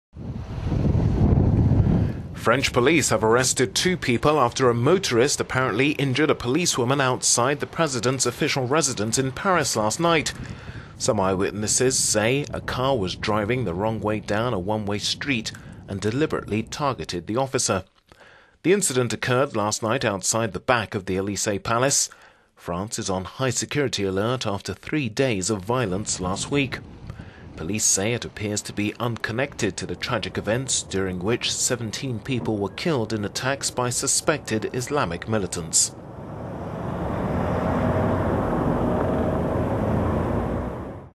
Two arrested after car hits French policewoman at Elysee Palace
French police have arrested two people after a motorist injured a policewoman outside President Francois Hollande's official residence in Paris.

Some eyewitnesses say a car was driving the wrong way down a one way street and deliberately targeted the officer.

The incident occured last night outside the back of the Elysee Palace.  France is on high security alert after three days of violence last week.